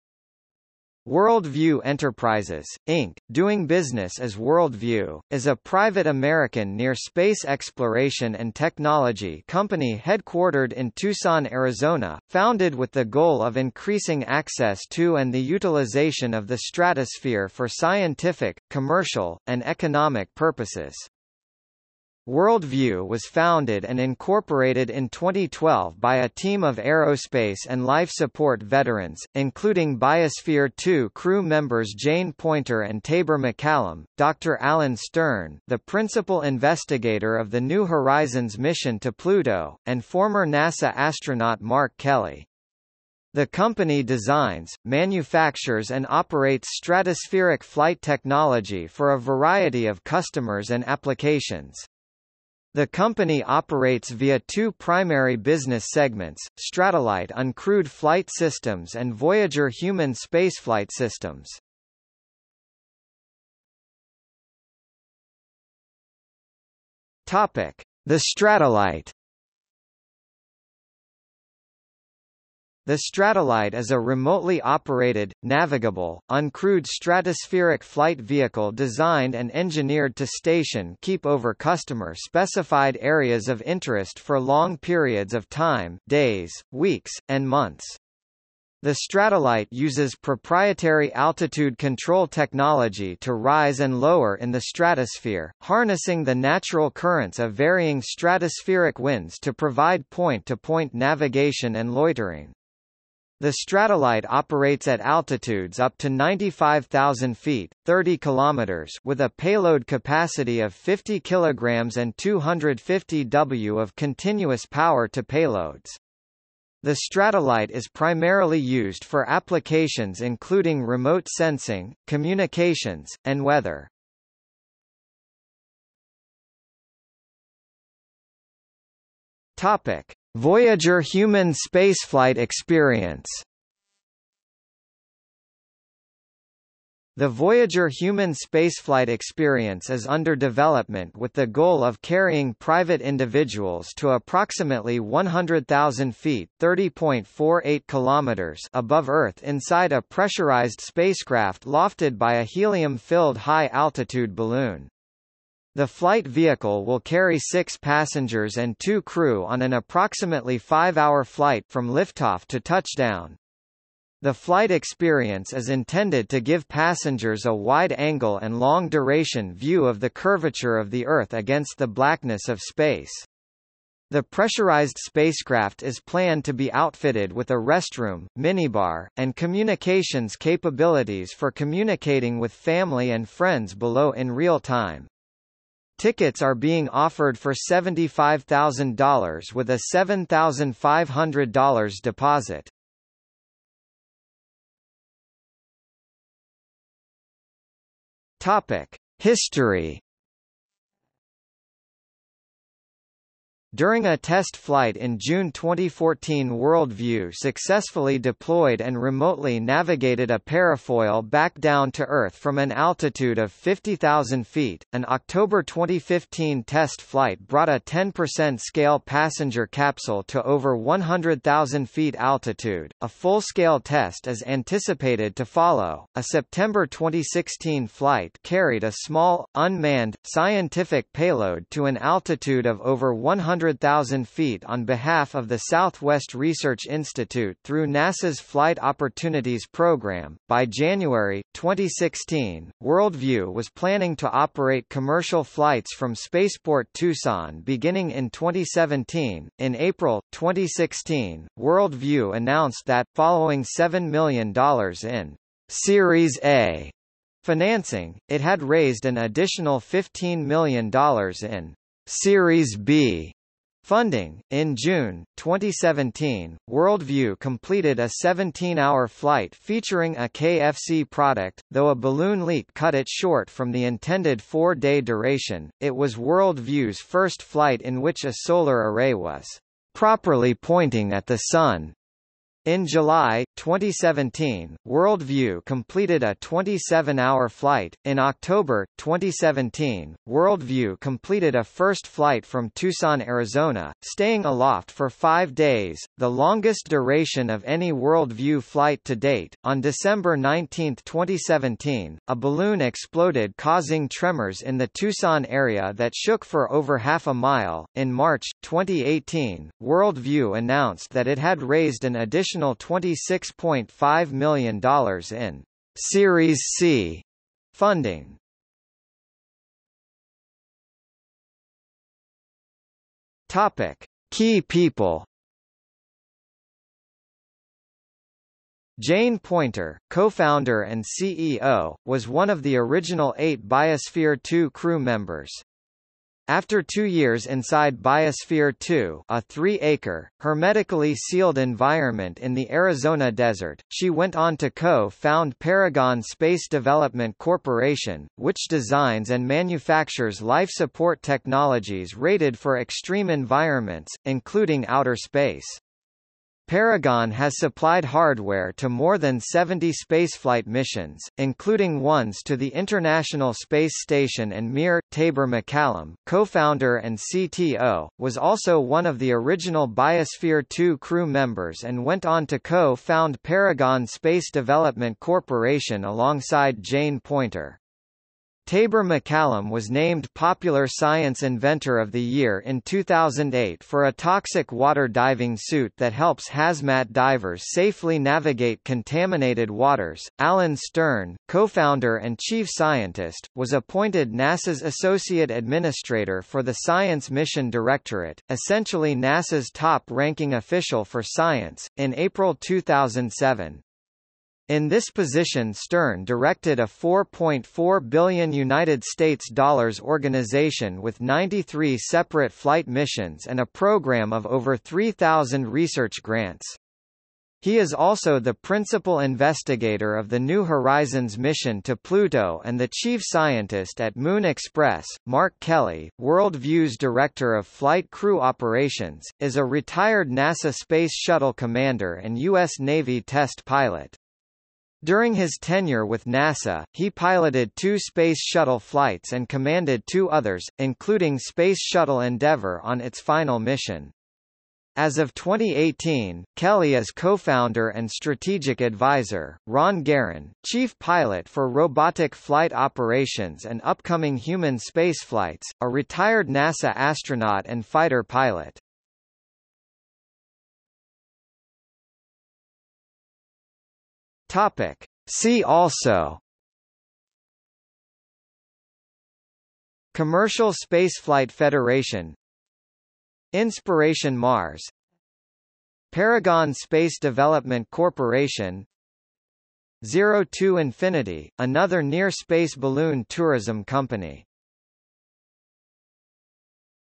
World View Enterprises | Wikipedia audio article
00:01:03 1 The Stratollite
00:02:05 2 Voyager Human Spaceflight Experience
00:03:14 3 History
00:05:19 4 Key people
00:07:40 5 See also

- Socrates

SUMMARY
World View Enterprises, Inc., doing business as World View, is a private American near-space exploration and technology company headquartered in Tucson, Arizona, founded with the goal of increasing access to and the utilization of the stratosphere for scientific, commercial, and economic purposes.
World View was founded and incorporated in 2012 by a team of aerospace and life support veterans, including Biosphere 2 crew-members Jane Poynter and Taber MacCallum,  Dr. Alan Stern (the principal investigator of the New Horizons mission to Pluto), and former NASA astronaut Mark Kelly. The company designs, manufactures and operates stratospheric flight technology for a variety of customers and applications. The company operates via two primary business segments: Stratollite un-crewed flight systems and Voyager human spaceflight systems.